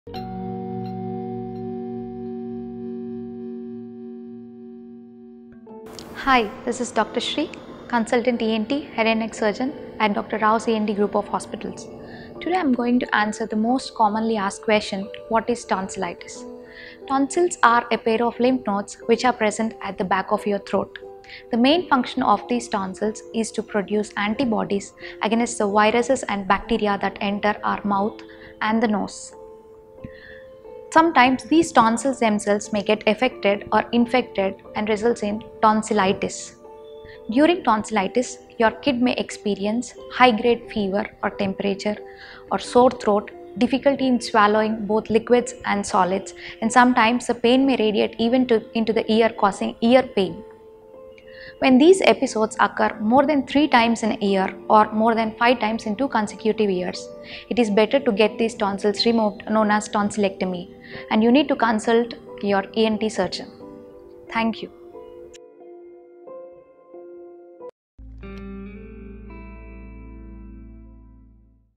Hi9 | What is Tonsillitis | Dr.C.Shree | ENT Surgeon
What is Tonsillitis | Hi9 | Dr. Rao's Ent Super Speciality International Hospital – Tonsillitis are a pair of lymph node which are present at back of your throat, the main function of this tonsils is to produce anti bodies against viruses and bacteria that enter our Mouth and Nose. Some times this tonsils themselves may get effected or infected and results in Tonsillitis, during tonsillitis your kid may experience high great fever or temperature, swore throat, difficulty in swallowing both liquids and solids.